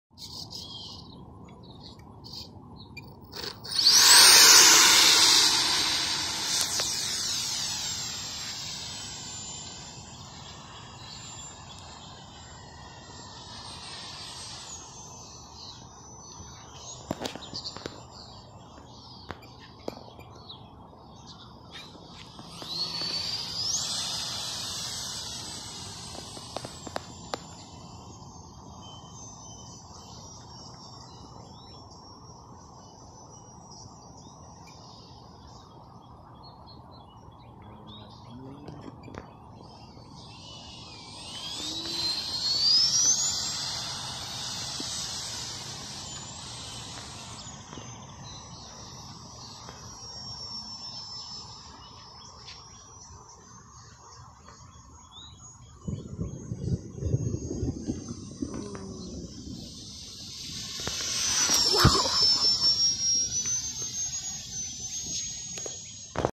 Freewing F-22 64mm on 6S!
Changed out the ESC and motor and now she really moves!  Happy July 4th